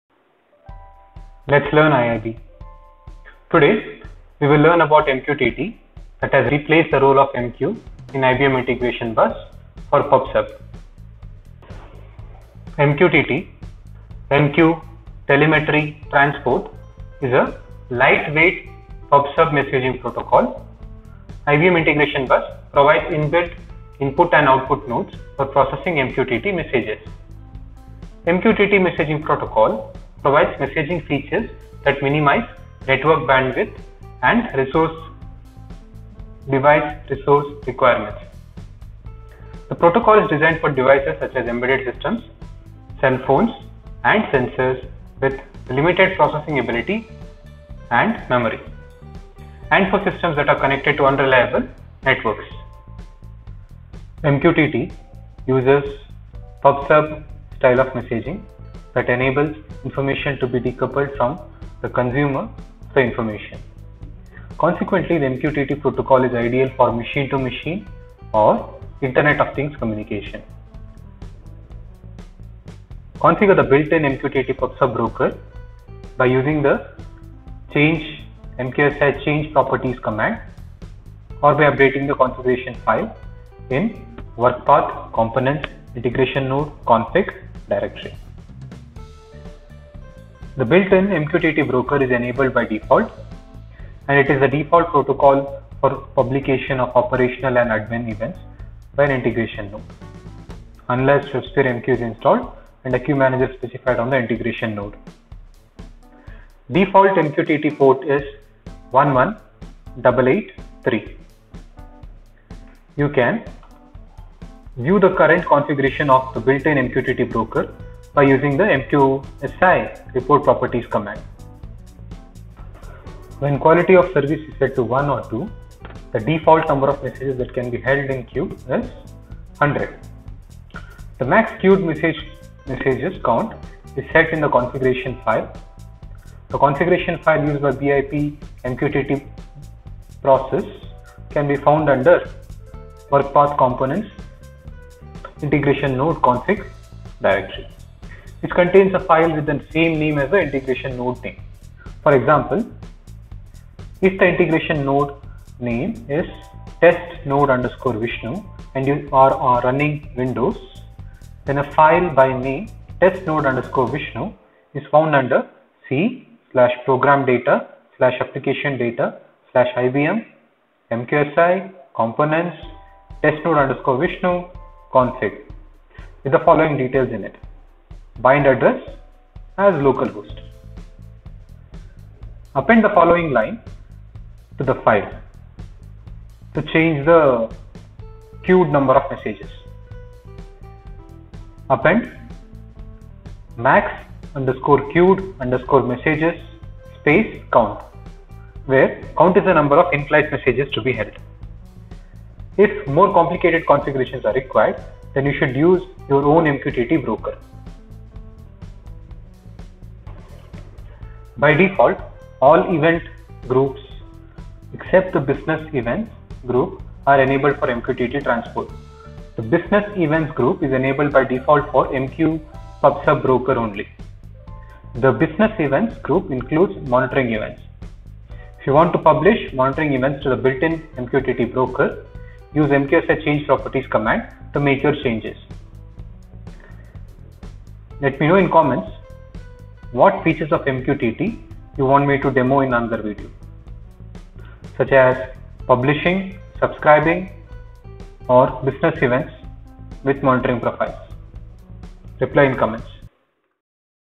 iib - mqtt broker for publish and subscribe - IBM Integration Bus
Let's learn IIB
ibm integration bus, ibm integration bus training, ibm integration bus tutorials, websphere message broker

Today we learn about mqtt that has replaced the role of mq in iib for pub sub.

Related Content : monitoring events & monitoring profiles with mqtt broker